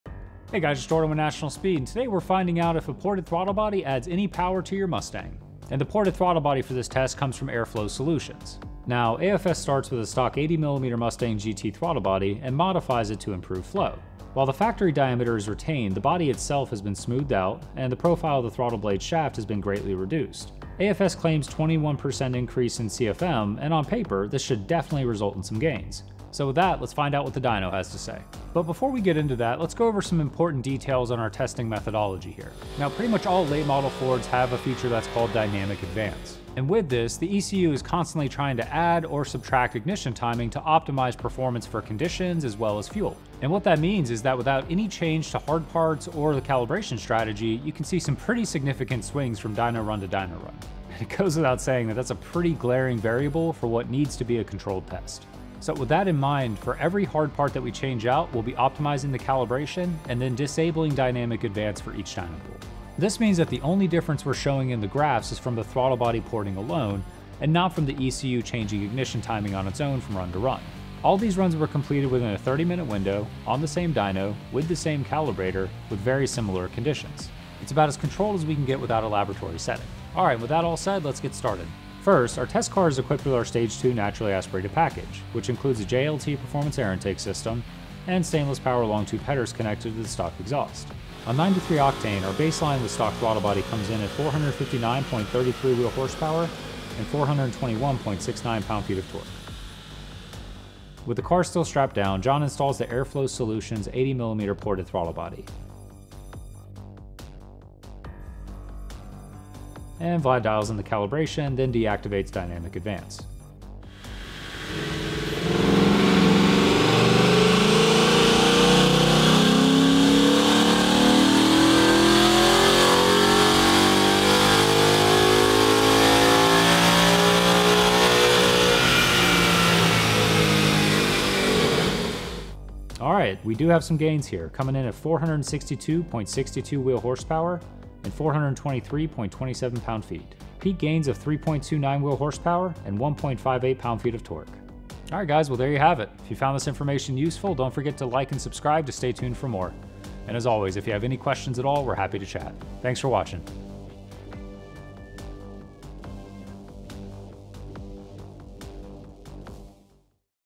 DYNO TESTED: Ported Throttle Body For Mustang Worth It?
Ported throttle body is another one of those common modifications for Ford Mustang.  And while it's super common, there's to be little consensus in terms of real gains.  Some claim 5hp gains, others claim 20hp, and some say it's useless.  So, we decided to do a controlled test and find out the right way!